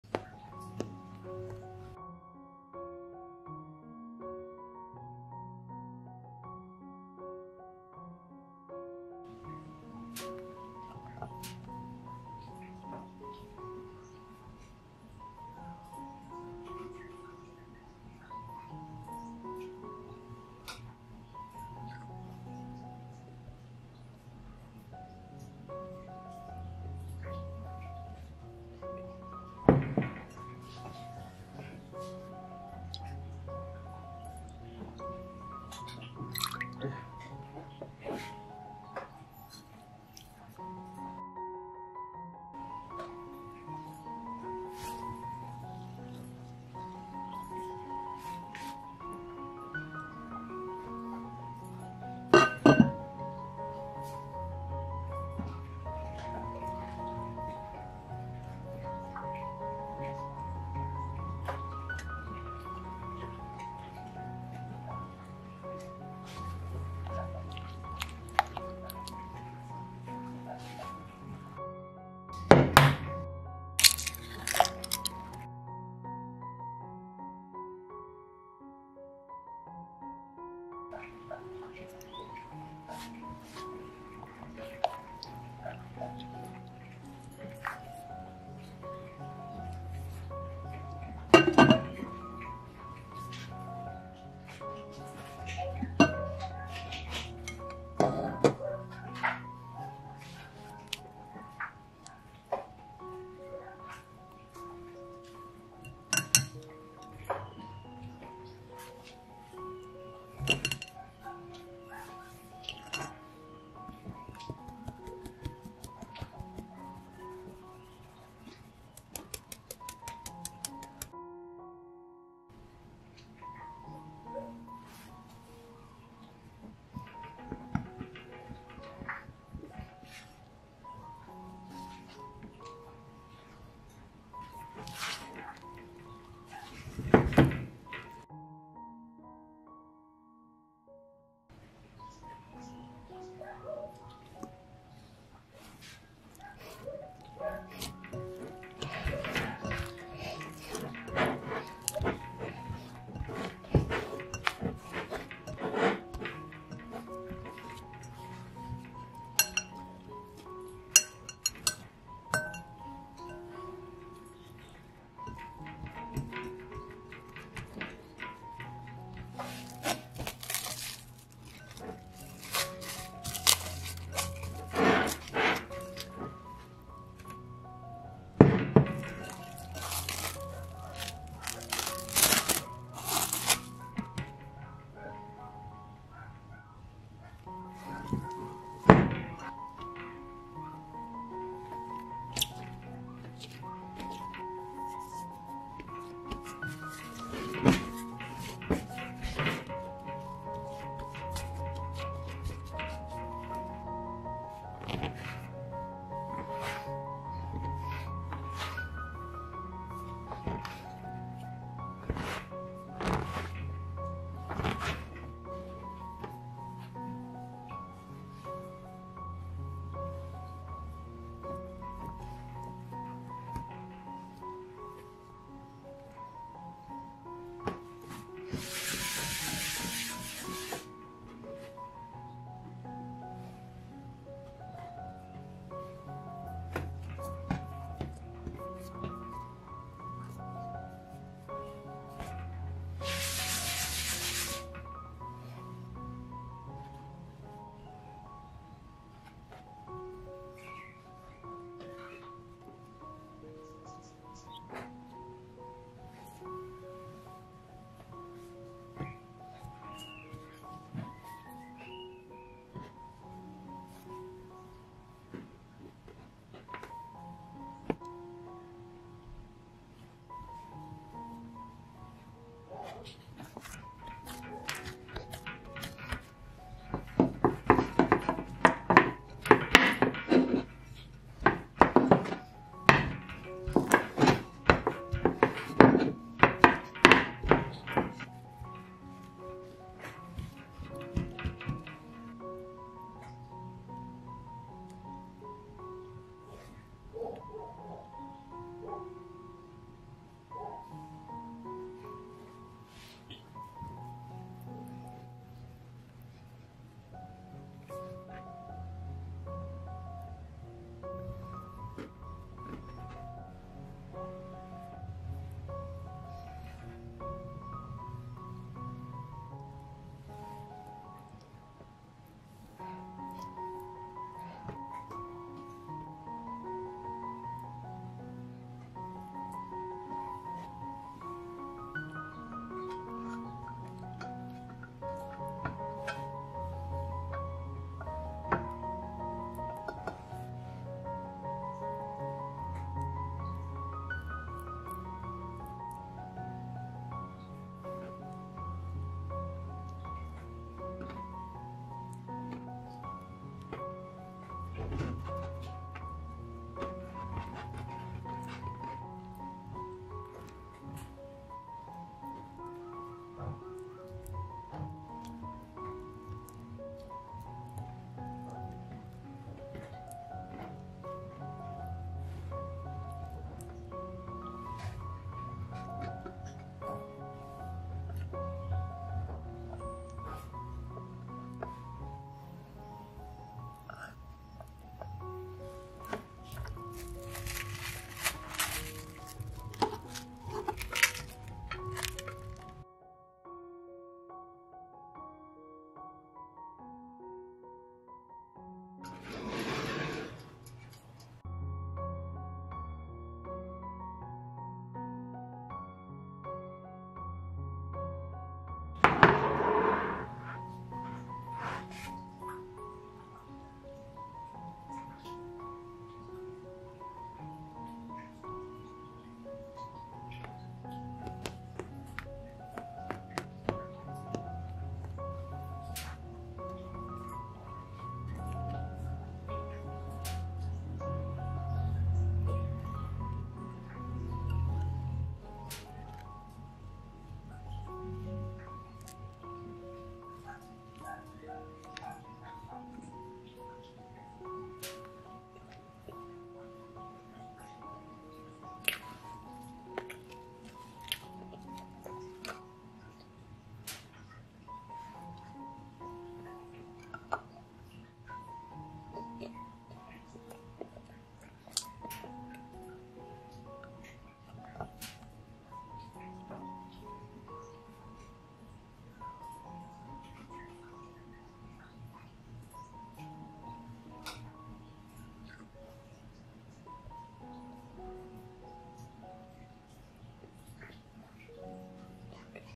Strawberry jam bread roll recipe | Bread rolls with jam | Bloom
_____Recipe______
300g Bread Flour
150ml Milk
6g Dry Yeast
25g Sugar
1 tsp Salt
1 tsp Vanilla
1 Egg
20g Butter
Strawberry jam

5 minute starter
Incubate 15 minutes
1st time Incubated for 1 hour
2nd time Incubate 15 minutes
3rd time Incubate 30 minutes

Bake 170°C for 25 minutes